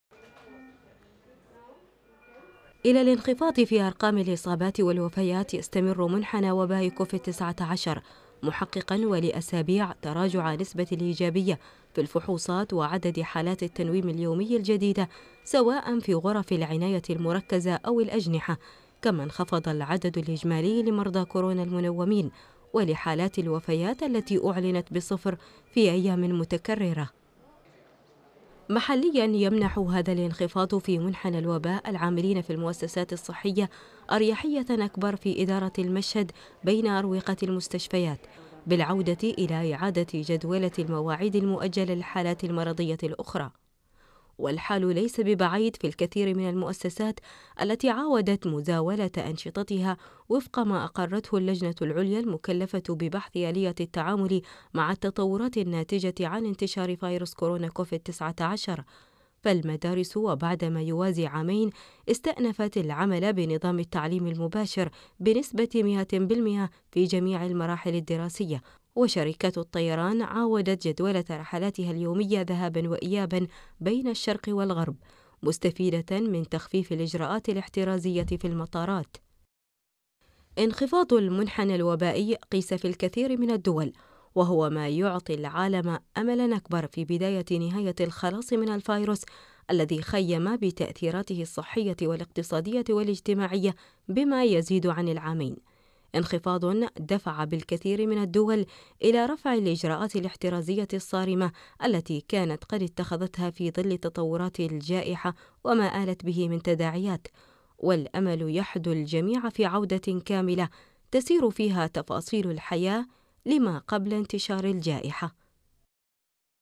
منحنى أرقام فيروس كورونا (كوفيد-19) يواصل الانخفاض دون تسجيل وفيات
منحنى أرقام فيروس كورونا (كوفيد-19) يواصل الانخفاض دون تسجيل وفيات تقرير إنتصار بنت حبيب الشبلية